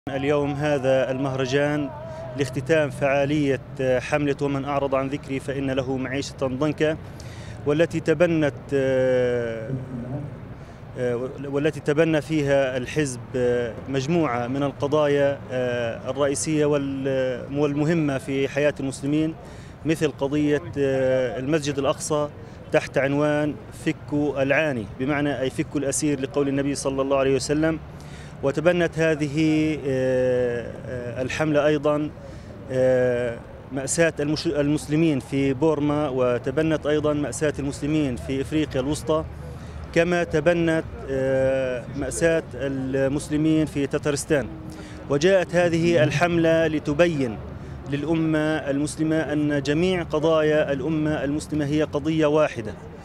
حملة "ومن اعرض عن ذكري فإن له معيشة ضنكا"
أقام حزب التحرير مساء السبت مهرجاناً خطابياً في حي نزال، بمناسبة الذكرى التسعين لهدم دولة الخلافة بحسب التقويم الميلادي، ومرور ثلاثة أعوام على انطلاق الثورة المباركة في الشام.